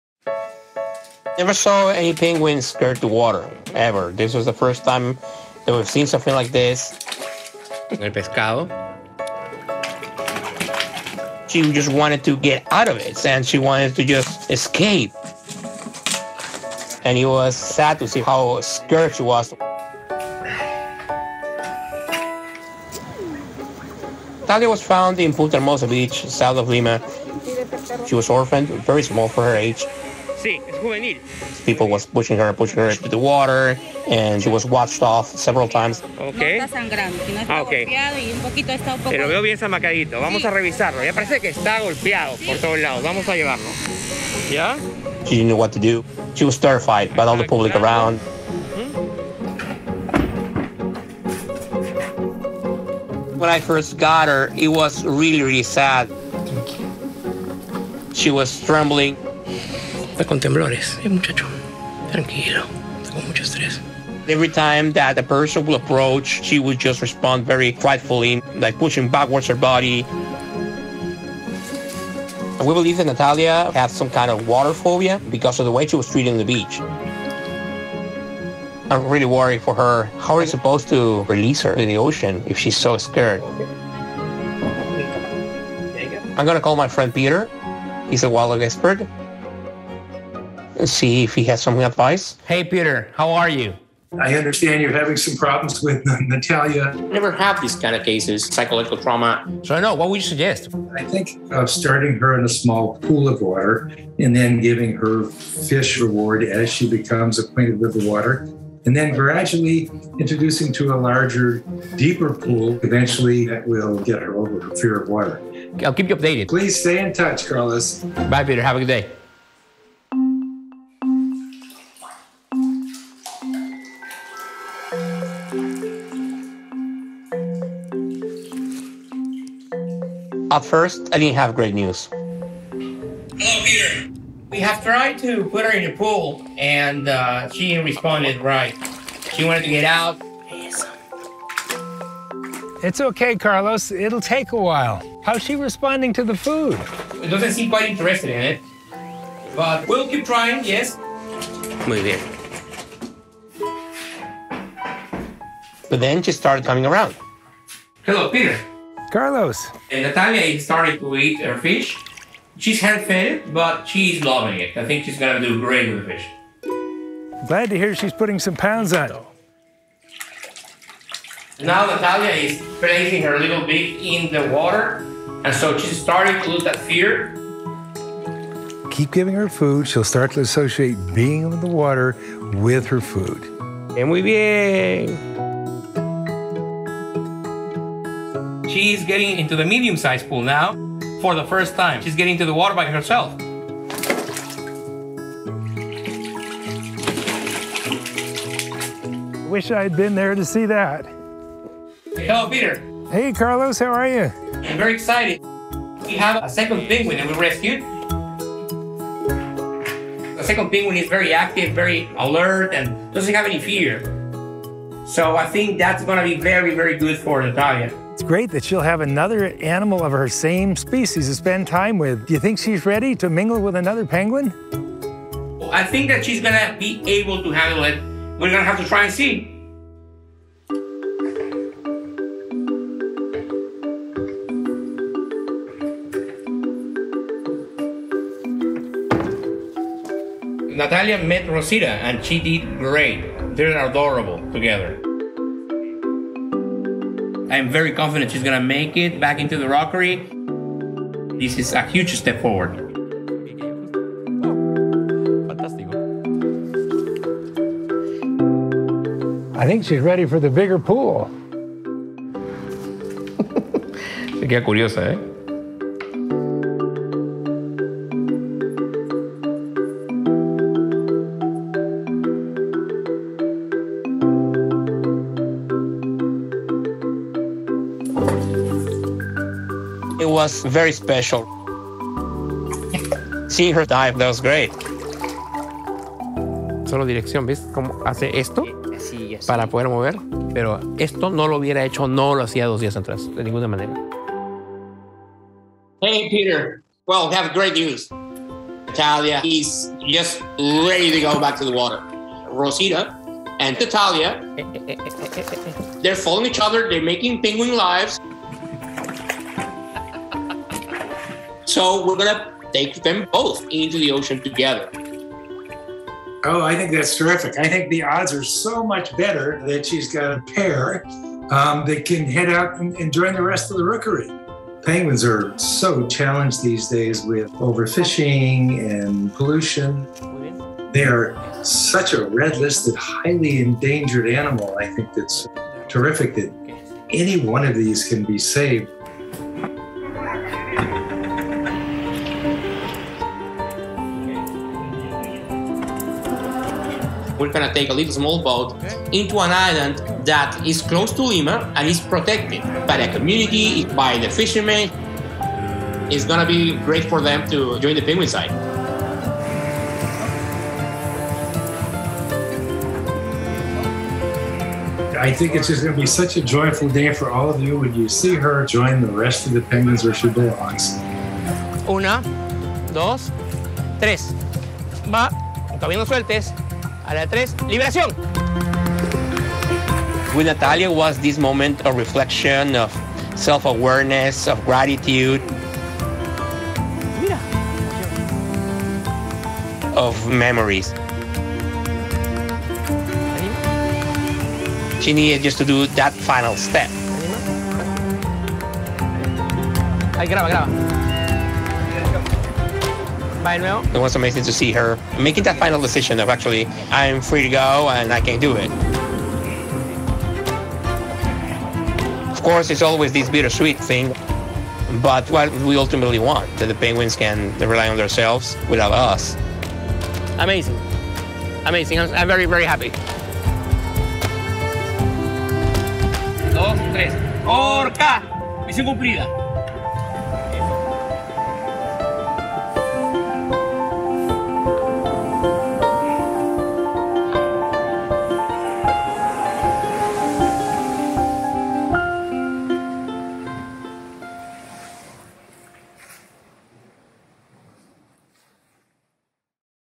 Watch This Guy Help A Baby Penguin Overcome Her Fear Of Water | The Dodo Saving The Wild
Orphaned baby penguin was terrified of water after tourists found her stranded and tried to throw her back into the ocean — watch this guy help her overcome her fear so she can be wild again 💙🥺

To learn more about Mutual of Omaha's Wild Kingdom Follow Wild Kingdom on Facebook YouTube and Instagram

To help more animals like Natalia, check out Orca Peru on Instagram